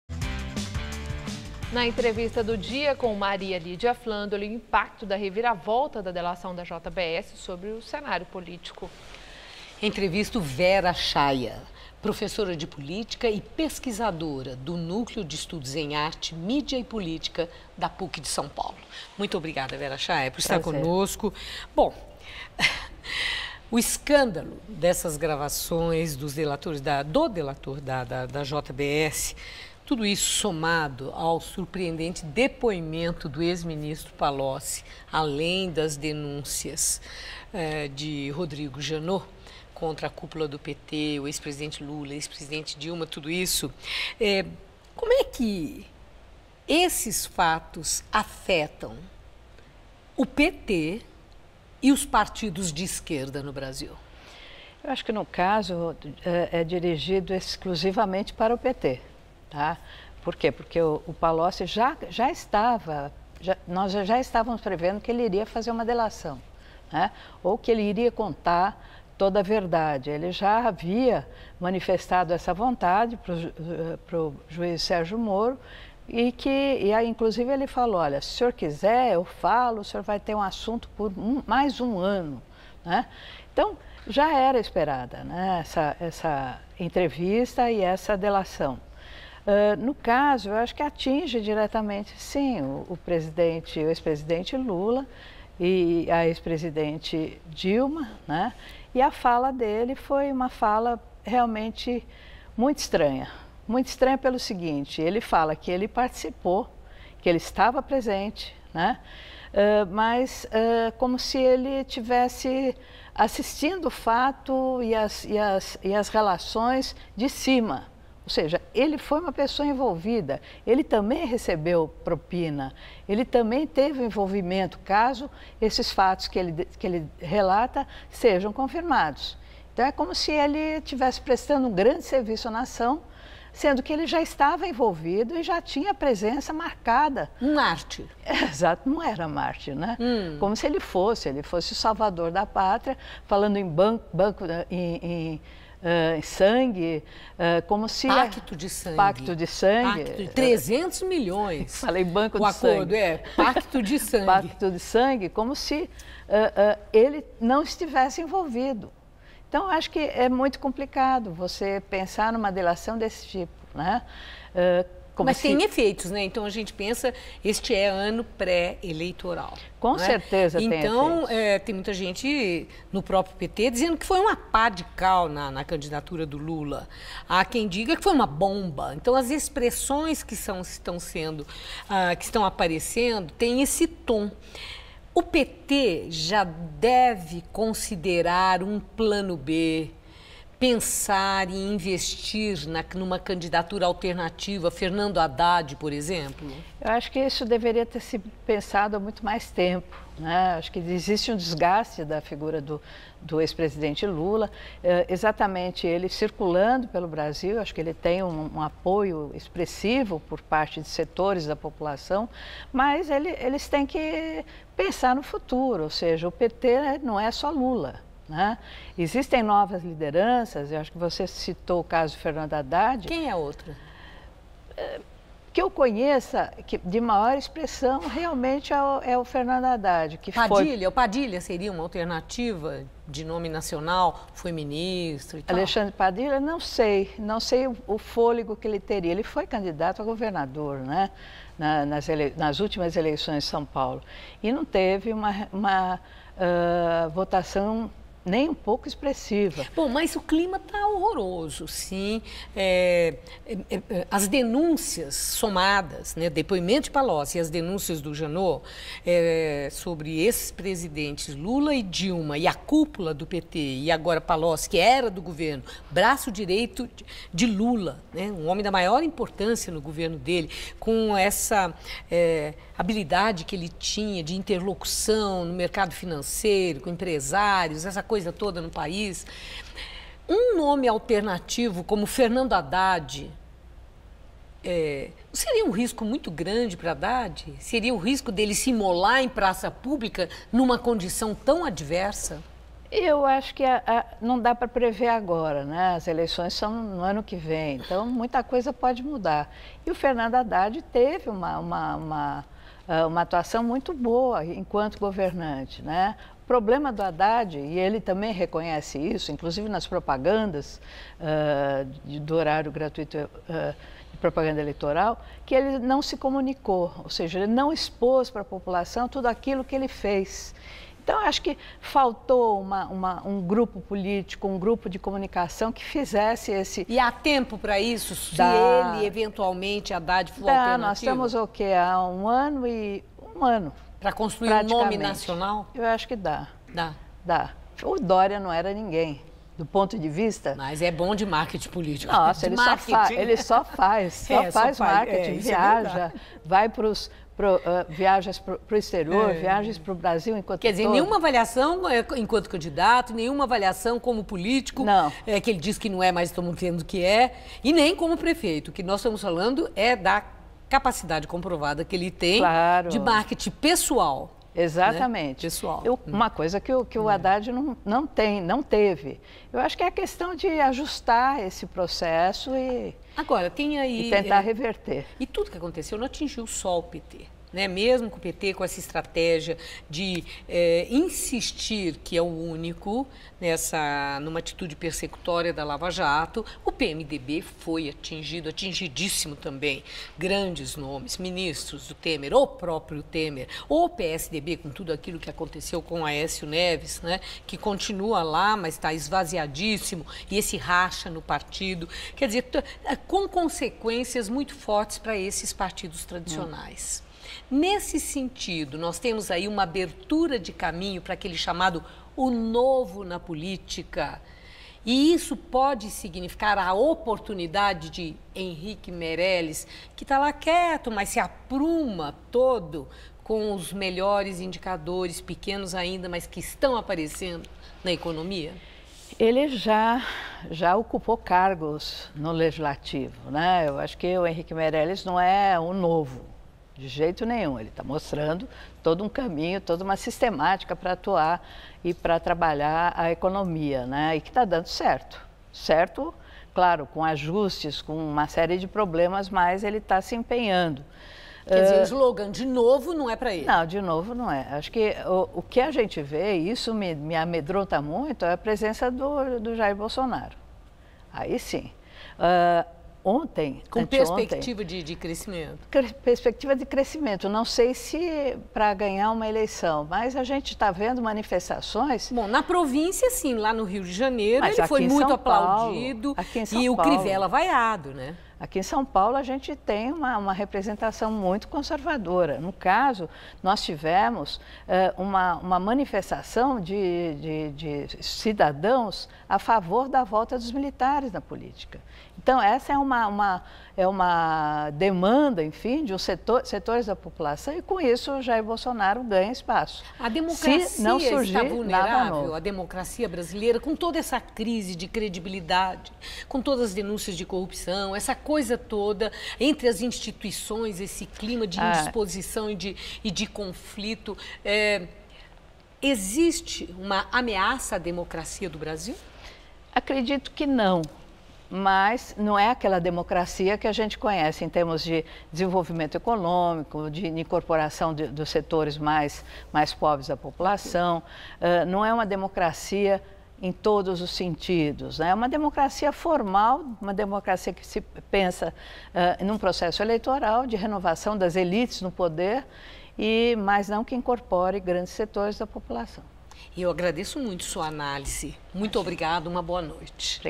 Maria Lydia entrevista Vera Chaia, professora e pesquisadora - PUC/SP, sobre a crise política
Na entrevista do dia com Maria Lydia Flândoli o impacto da reviravolta da delação da JBS sobre o cenário político.